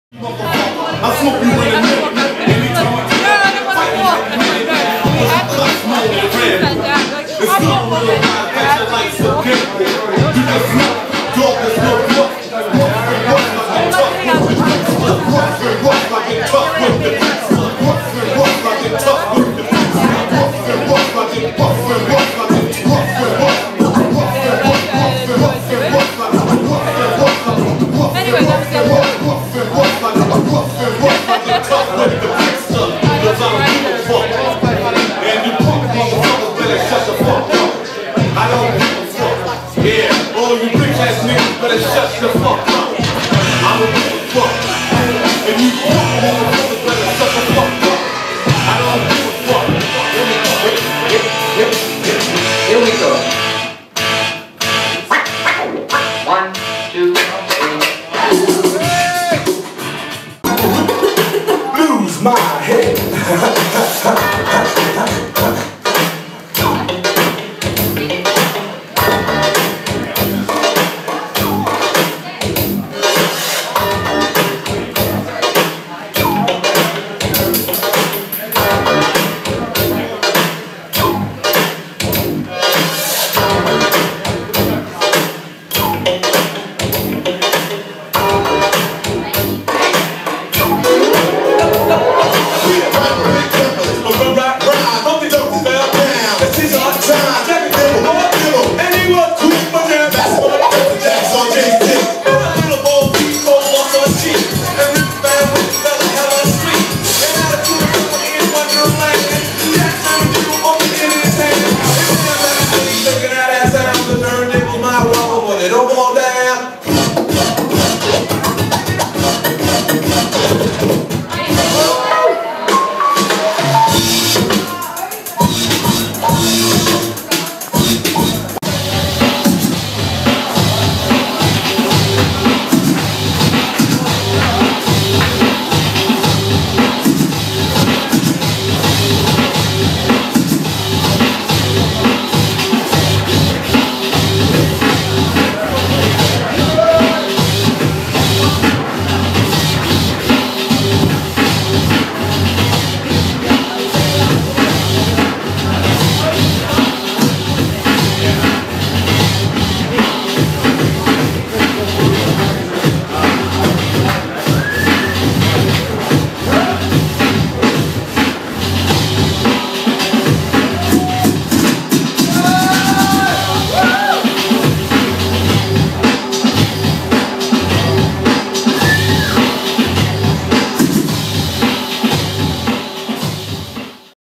Mark N @ &co (Sydney)
A few highlights from a 90 minute all hip-hop set.